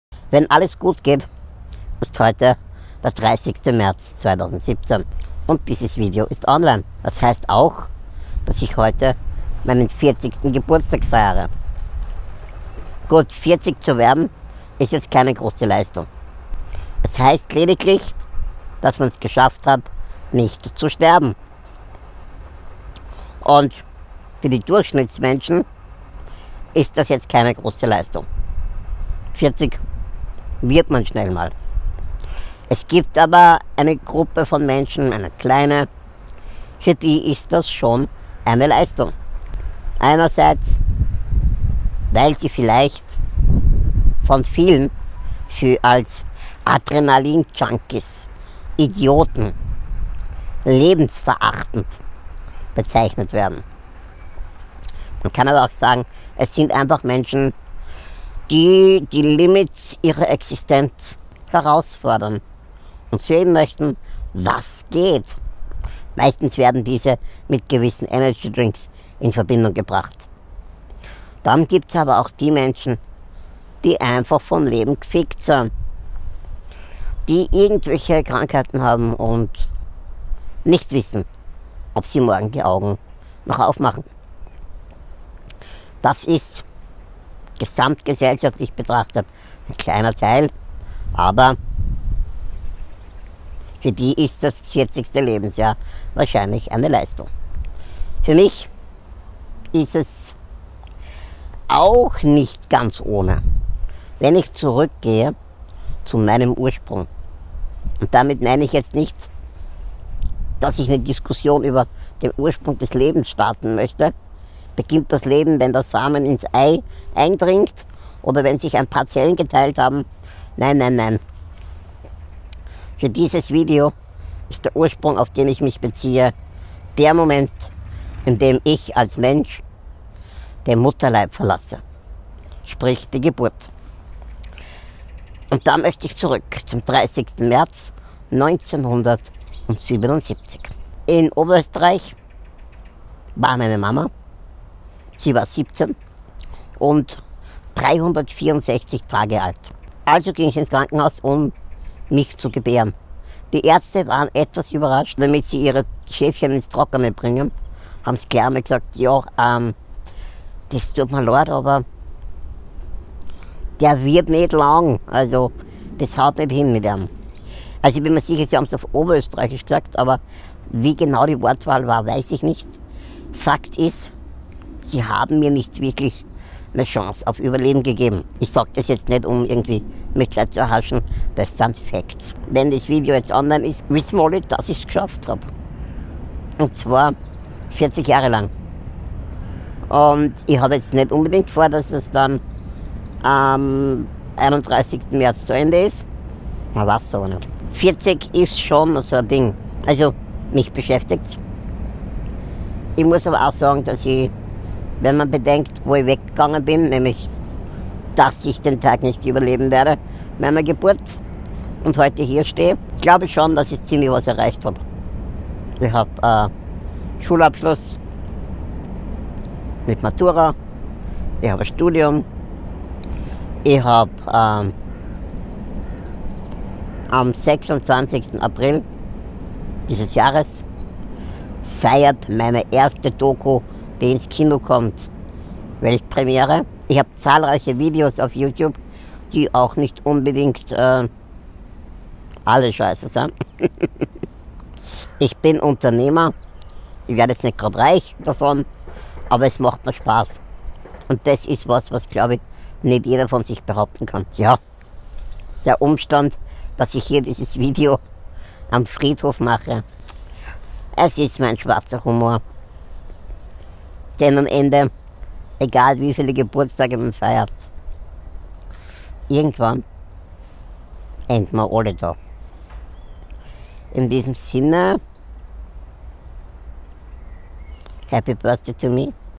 Mein 40. Geburtstag. 🎂 Ein kleines Fazit.
Heute feiere ich meinen 🎂  40. Geburtstag. 🎂  Zeit für ein kleines Fazit. Happy Birthday to me!